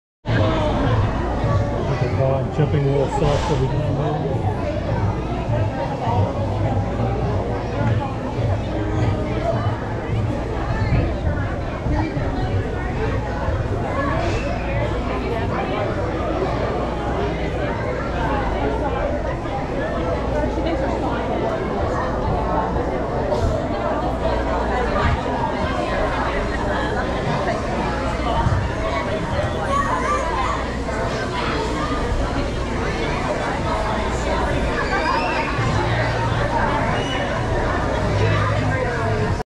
Flipping Around at Jumping World
Trying out gymnastic skills at Jumping World birthday party.